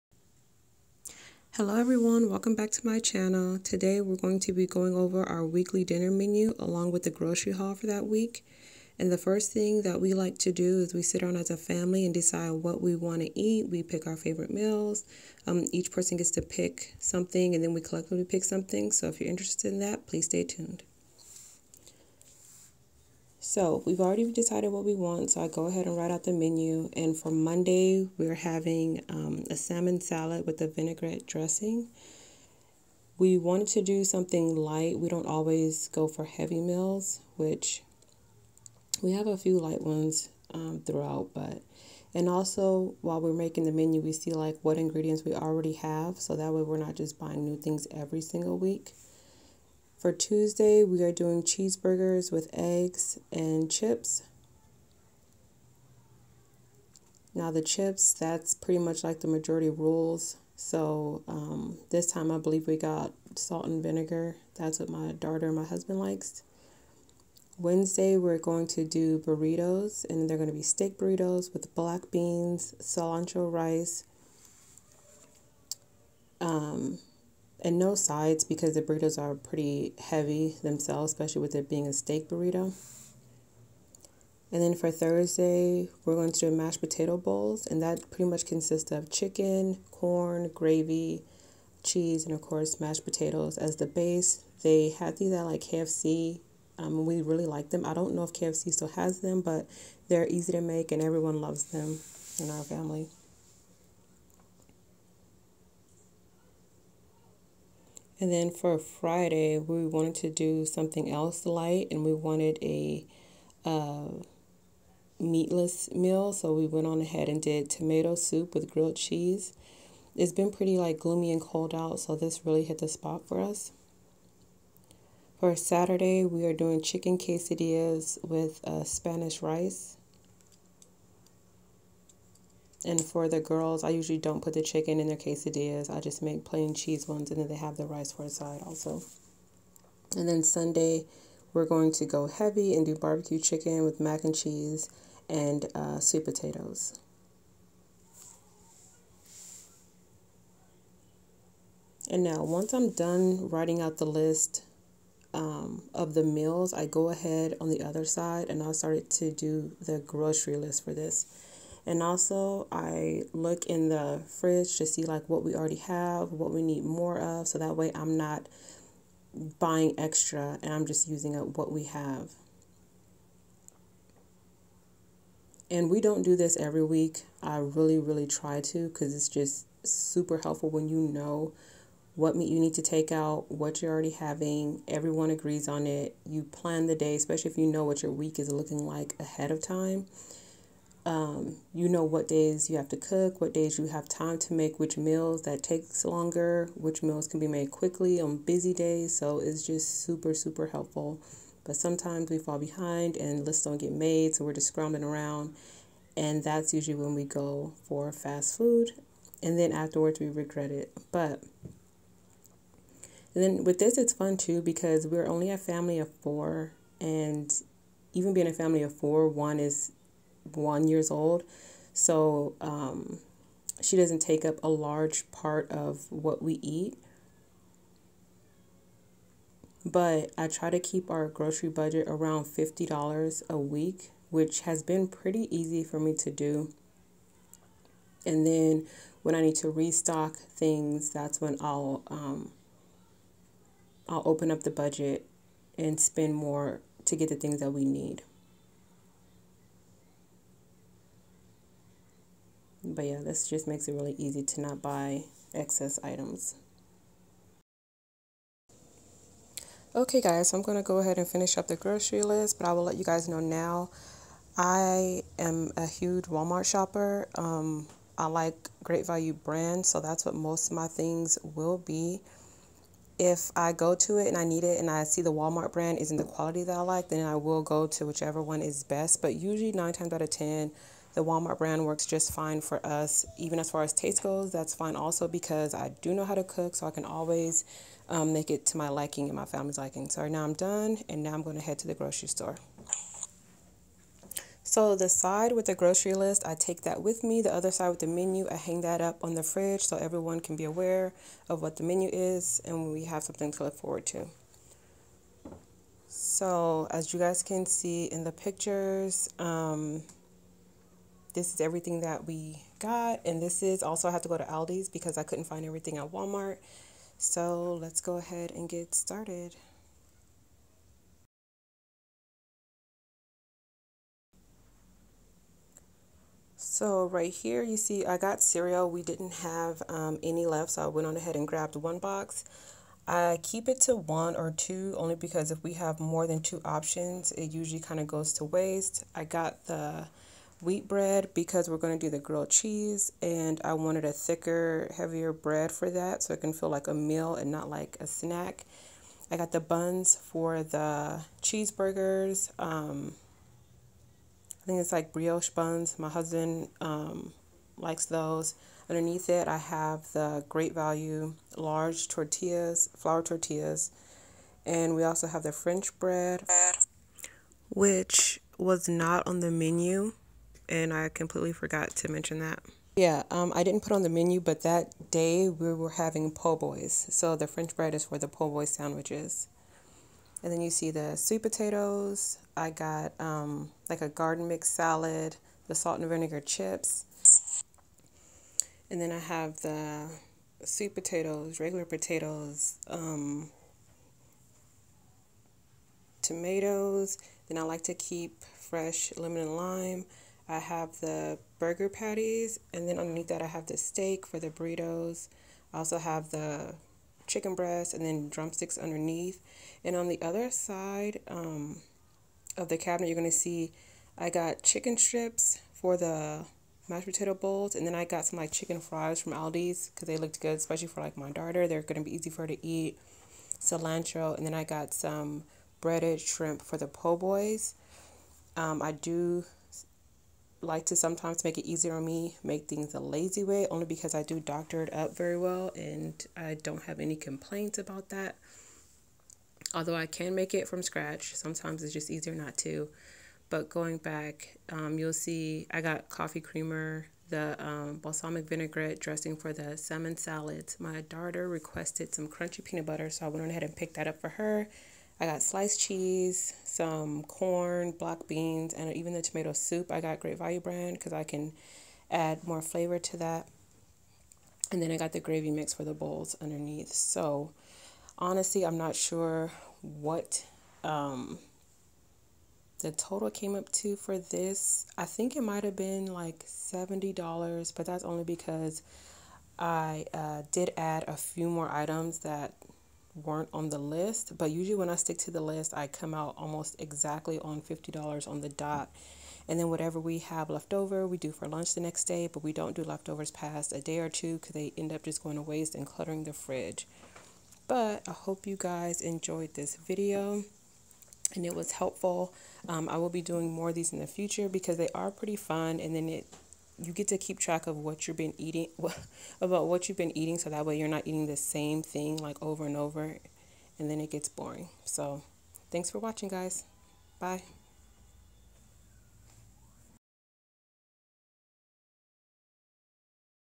Weekly Dinner Menu w/Grocery Haul
Our weekly routine for dinner planning along with a grocery haul. Doing our planning and preparing a week at a time allows us to not be overwhelmed while grocery shopping and prevents last minute fastfood runs lol